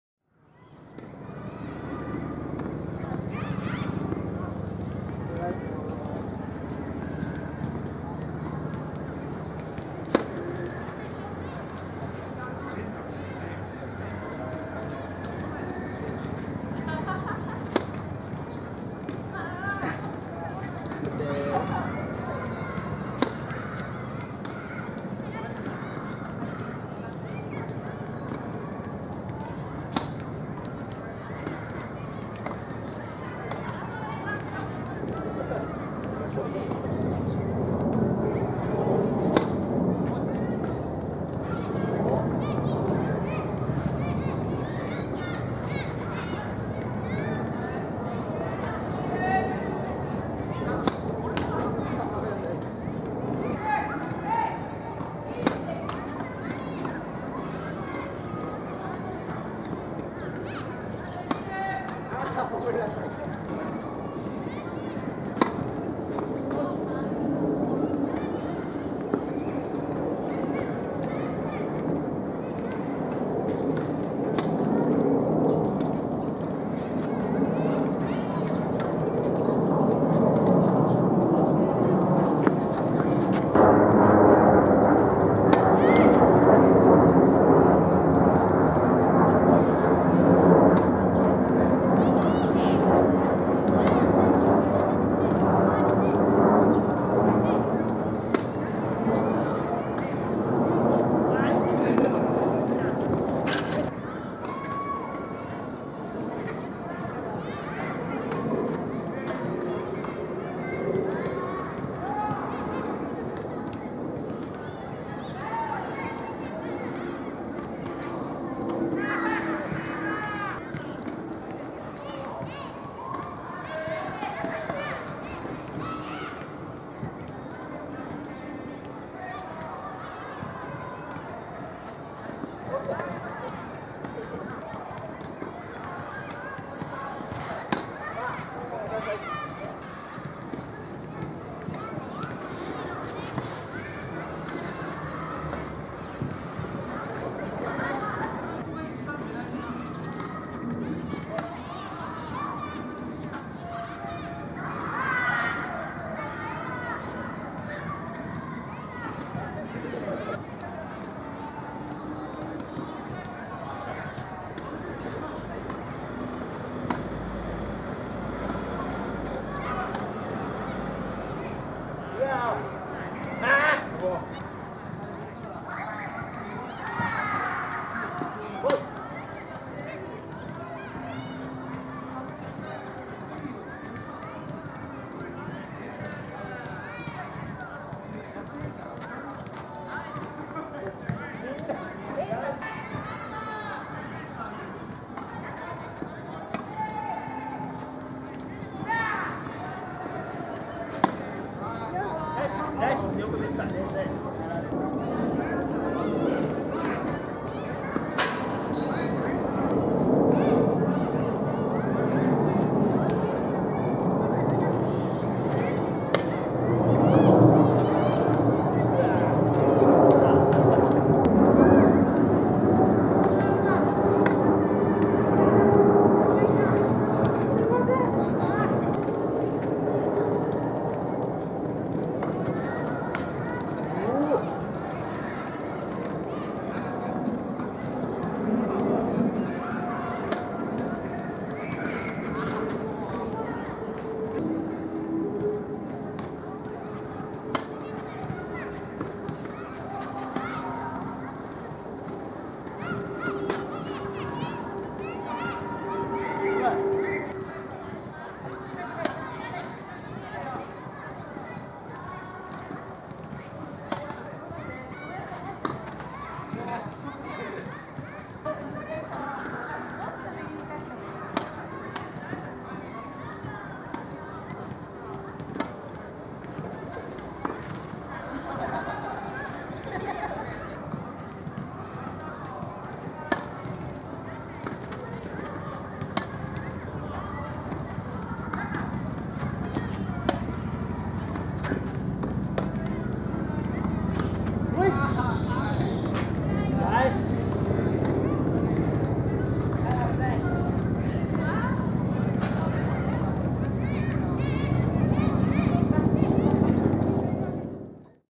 【渋谷区スポーツセンターでダブルスやりました！Ⅰ2022/03】
渋谷区のスポーツセンターで4ゲーム消化のダブルスやりました。（スコア入り）2022/03
【ひっくん チャンネルもよろしく！DIY・リフォーム等を中心の動画です！】